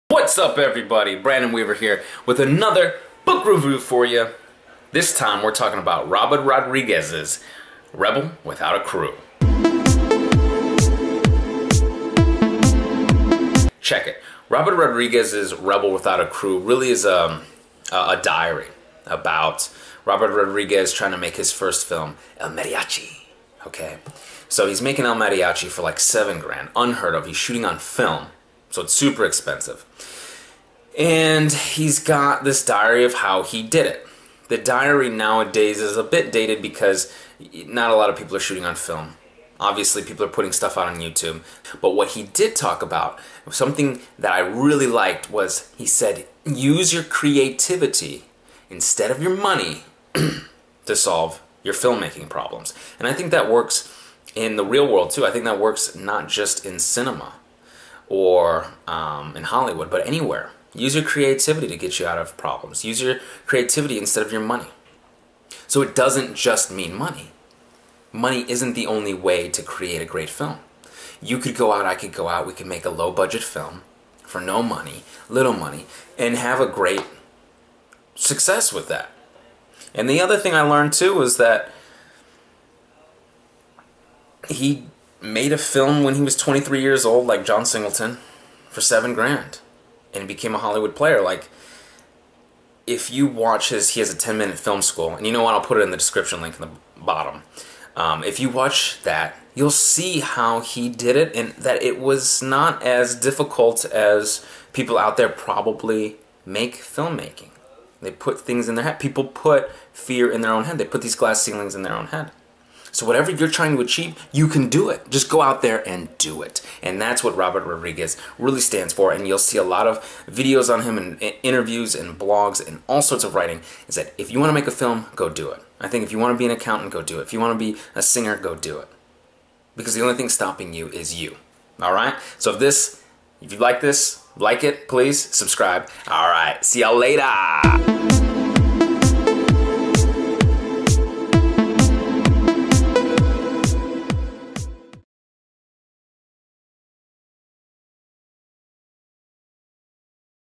How to Use Creativity not MONEY Robert Rodriguez
Robert Rodriguez's Rebel Without A Crew Book Review.

Robert Rodriguez made a film at 23 years of age and now owns his own tv network!  He claims Hollywood is there for the taking.  Go out and make your films.  Forget film school just watch how Robert Rodriguez makes  a film, read his book Rebel Without A Crew and go make a movie!